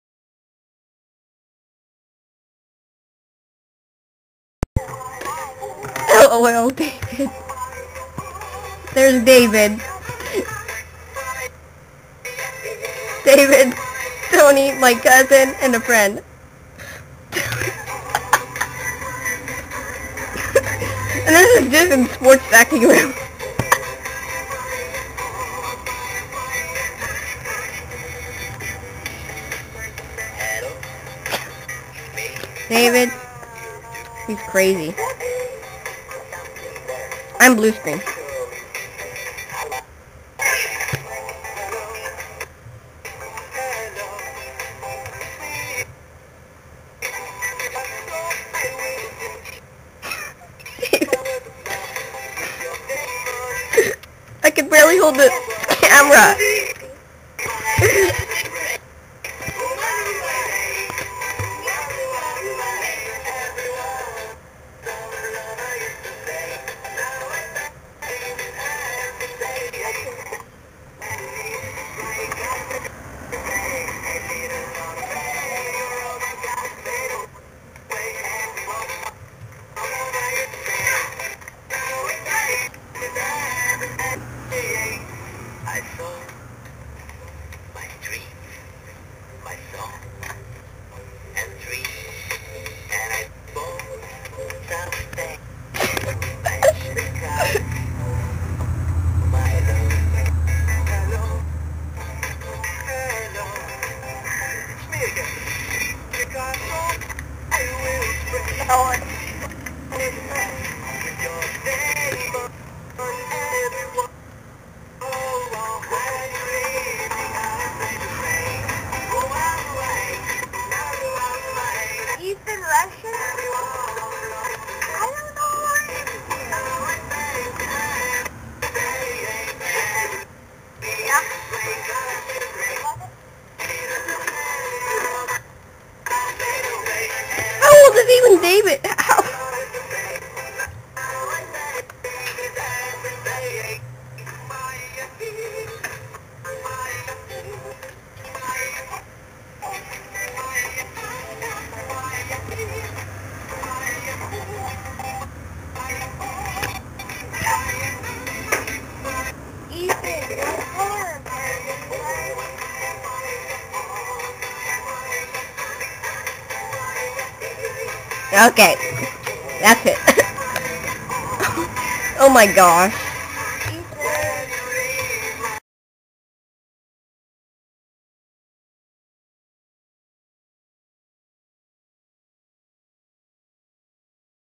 David on Mebeam
David on the chat rom at the sportstacking chatt room on Mebeam.com at the sportstacking chat room